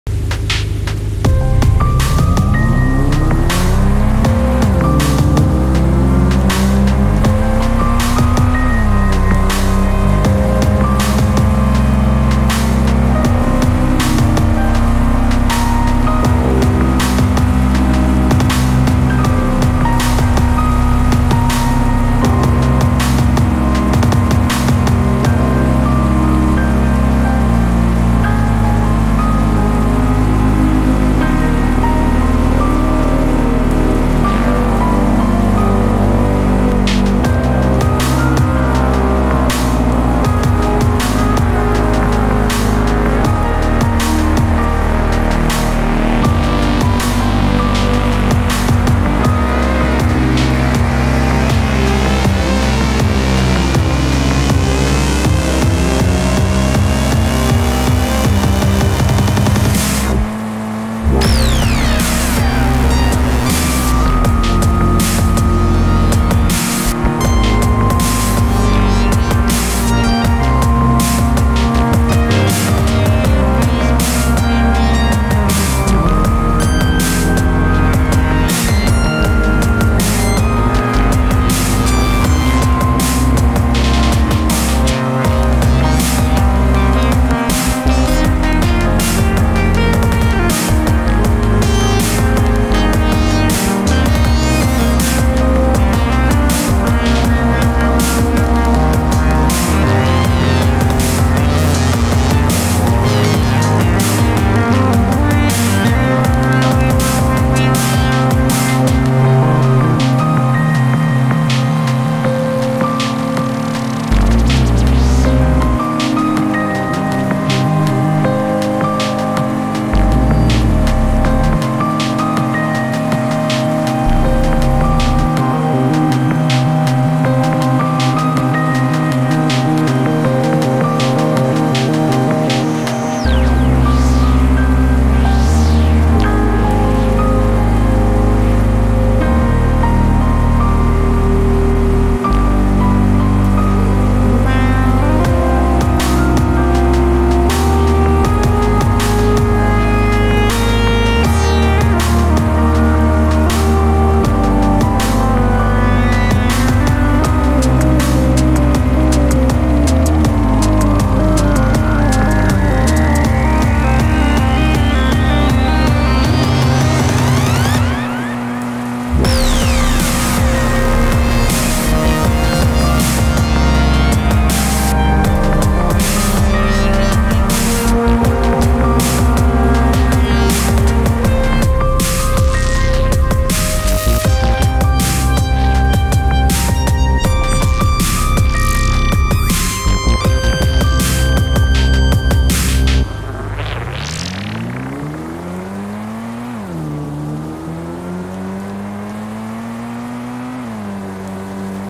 (Unity) Gran Turismo x DIRT Styled Indie Game | Devlog #2
Making a Gran Turismo x DIRT Styled game.
Messing Around with map design and new car.
Will be adding AI next